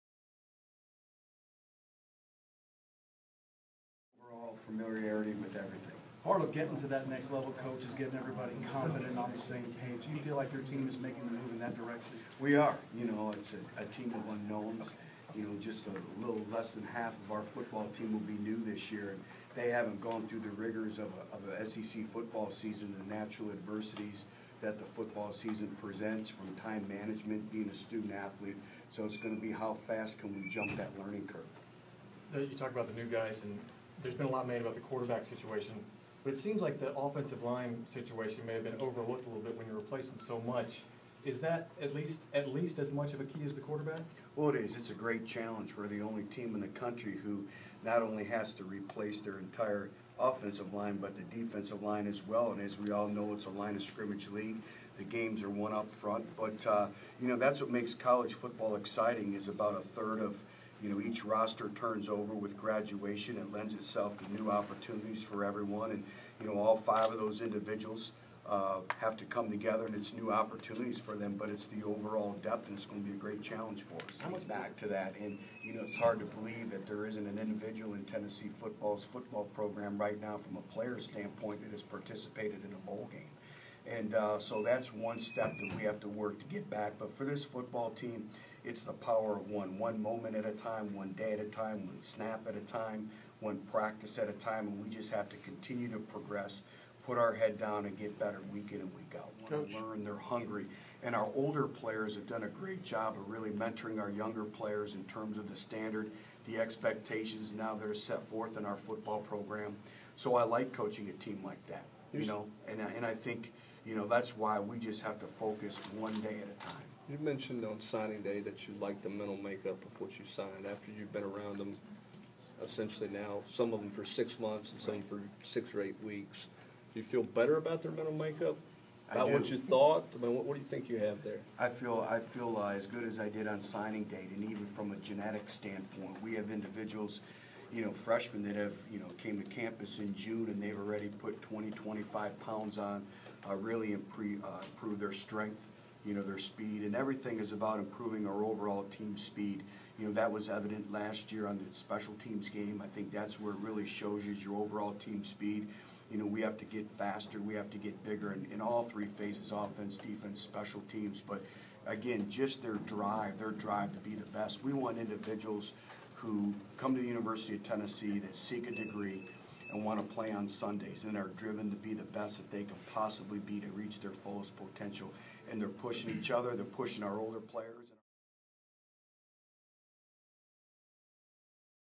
Tennessee's Butch Jones
Jones speaks to the media.